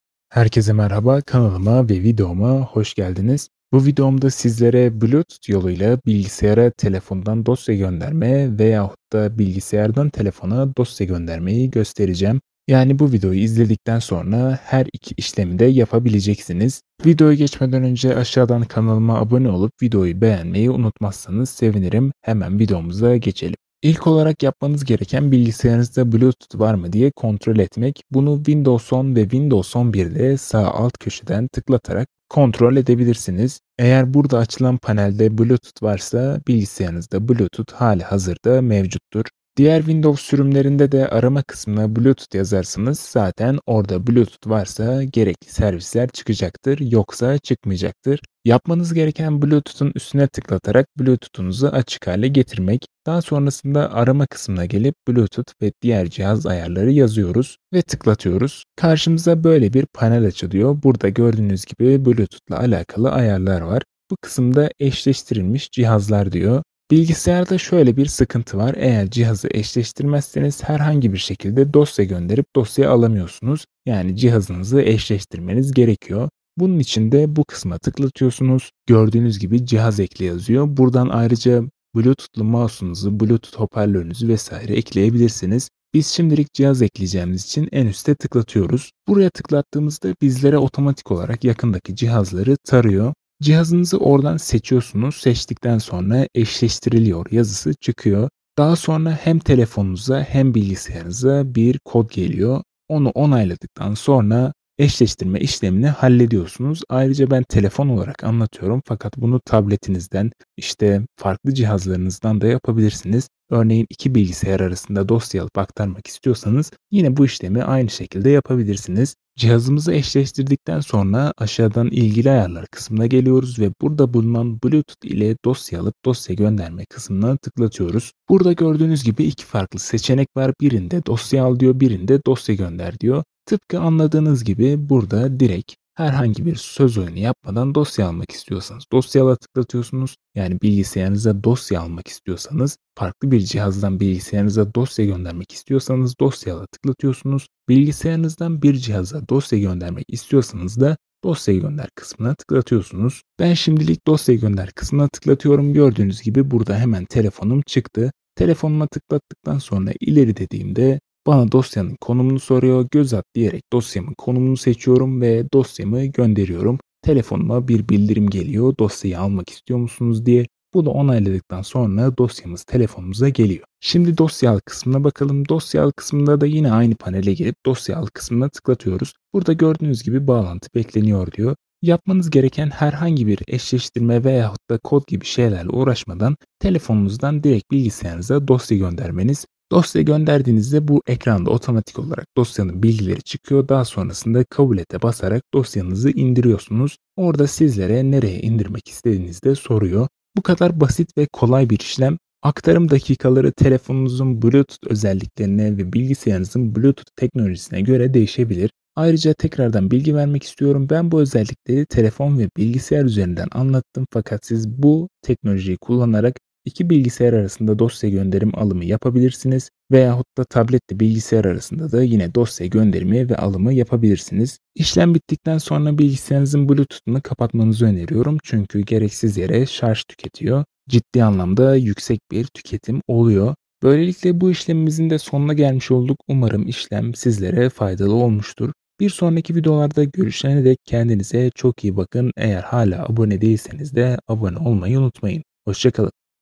Bilgisayarda Bluetooth Nasıl Kullanılır ? Bluetooth ile Dosya Gönderme-Alma Nasıl Yapılır?
Herkese merhaba, bu videomda sizlere bluetooth ile dosya alma ve dosya göndermeden bahsettim. Ayrıca bluetooth nasıl açılır bluetooth tuşu yok tarzı aramalar yapan arkadaşlar içinde faydalı bir içerik olacaktır.